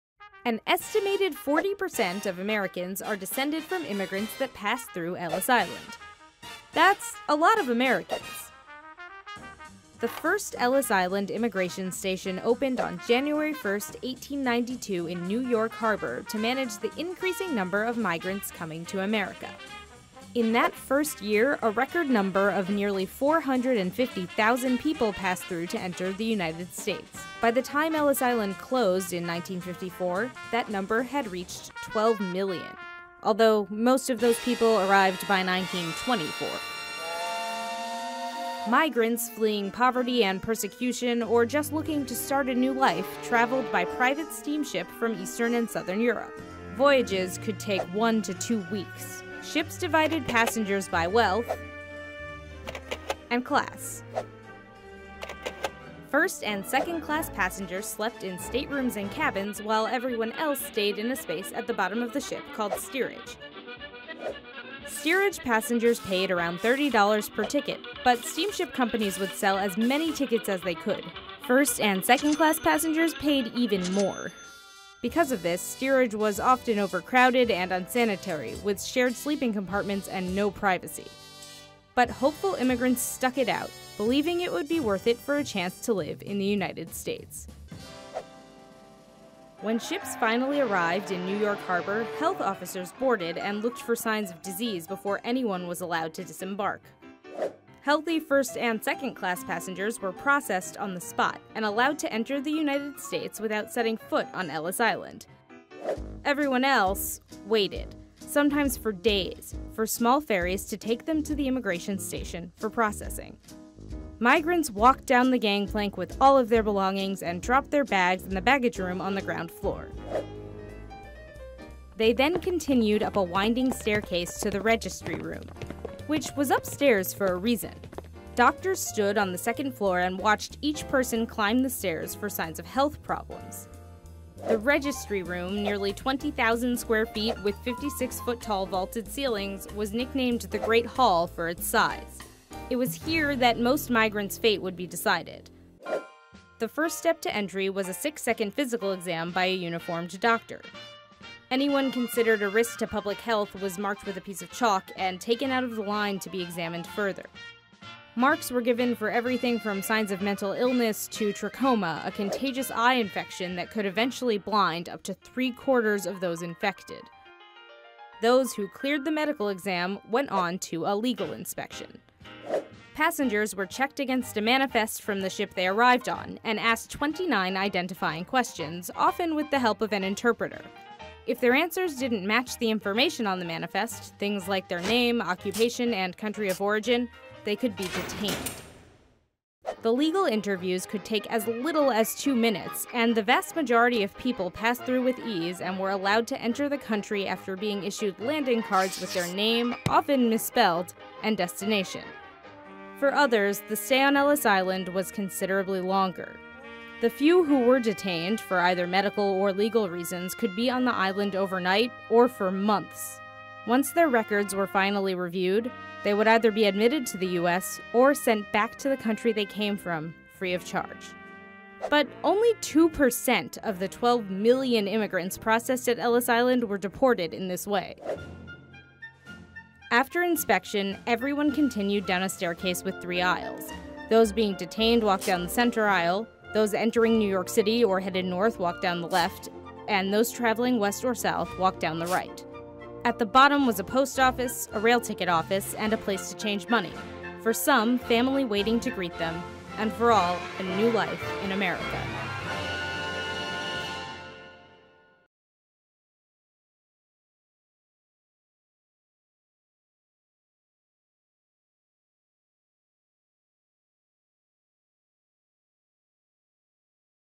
Ellis Island Historical site (Rising Immigration)
Most of the immigrants who came to America through Ellis Island were from eastern and southern Europe. In many cases, they came to escape the poverty and religious intolerance that existed in small towns in countries such as Italy, Poland, and Russia.
Estimated 40% of Americans are descended from people who passed through the Ellis Island immigration station during its six decades of operation. But what was the immigration process like?